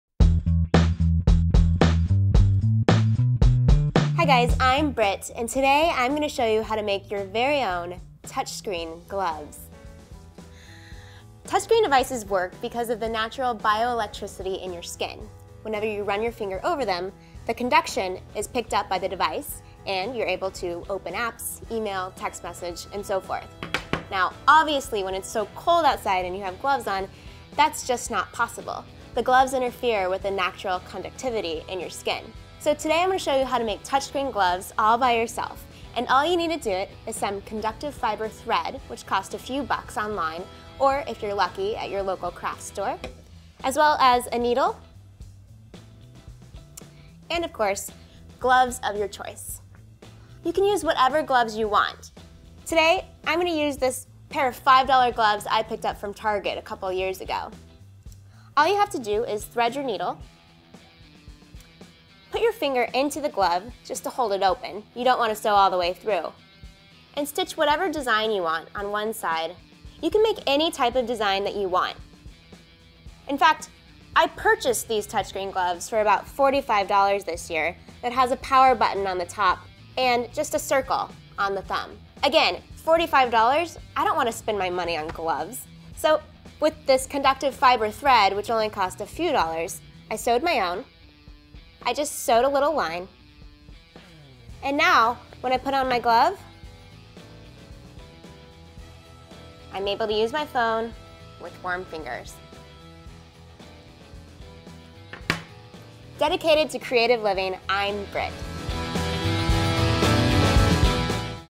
How to Make Touchscreen Gloves For Texting
Watch as Brit shows you how to make your own texting gloves by re-purposing old gloves and sewing patterns onto the fingertips using conductive fiber thread.